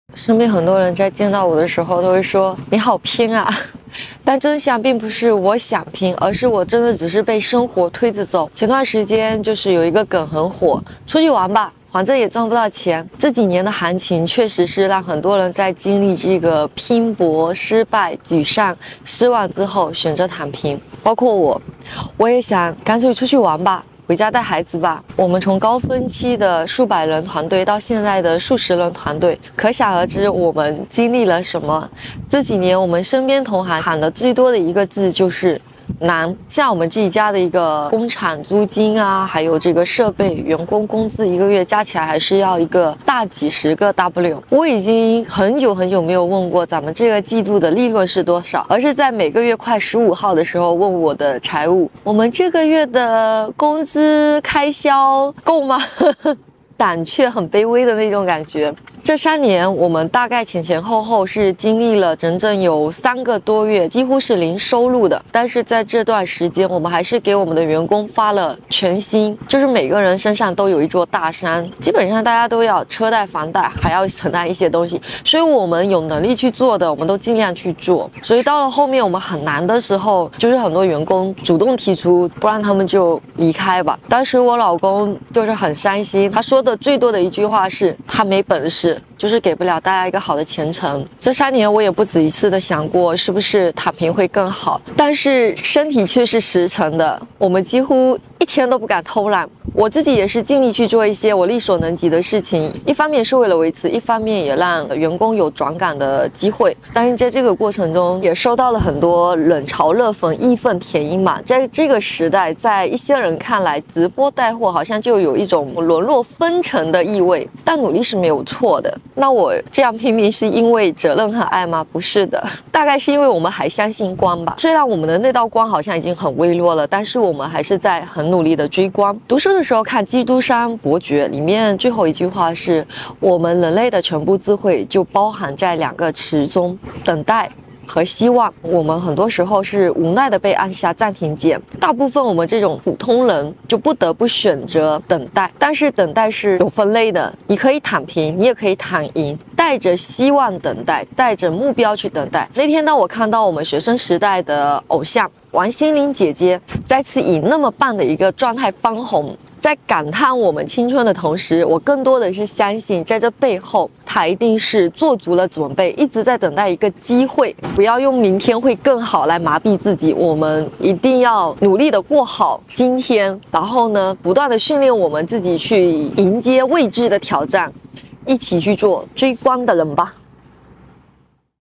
表嫂的百人團隊如今不到十人，員工紛紛主動辭職，表哥自責沒本事
未來的日子裡，讓我們做那個勇敢的追光人